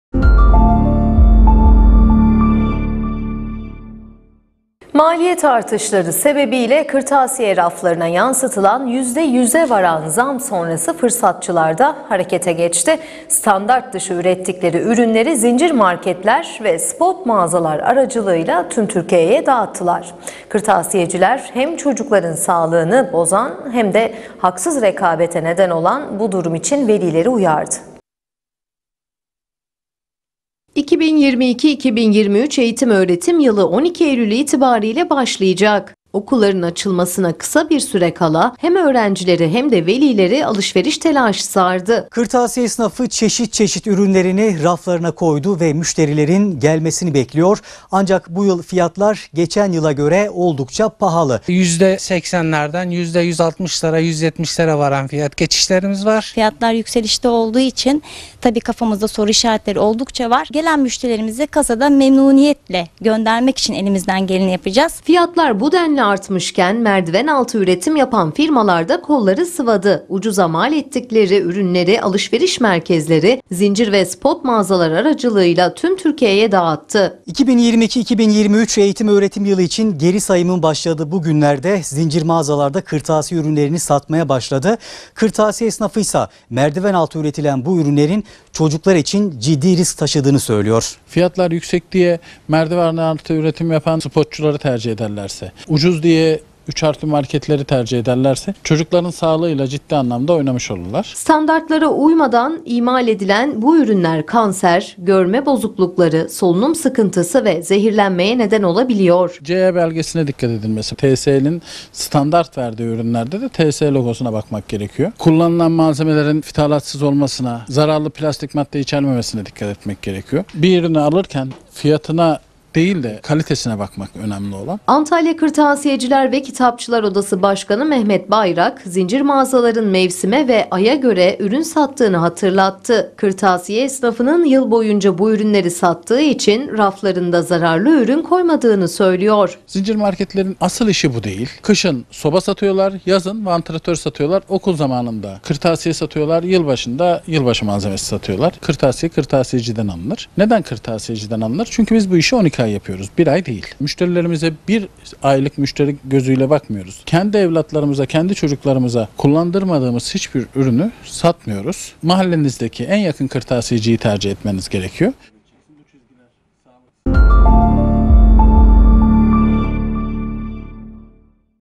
KIRTASİYE ÜRÜNLERİNE GELEN ZAM FIRSATÇILARA YARADI
MALİYET ARTIŞLARI SEBEBİYLE KIRTASİYE RAFLARINA YANSITILAN YÜZDE 100'E VARAN ZAM SONRASI FIRSATÇILAR DA HAREKETE GEÇTİ. STANDART DIŞI ÜRETTİKLERİ ÜRÜNLERİ ZİNCİR MARKETLER VE SPOR MAĞAZALAR ARACILIĞIYLA TÜM TÜRKİYE'YE DAĞITTI. KIRTASİYECİLER, HEM ÇOCUKLARIN SAĞLIĞINI BOZAN HEM DE HAKSIZ REKABETE NEDEN OLAN BU DURUM İÇİN VELİLERİ UYARIYOR.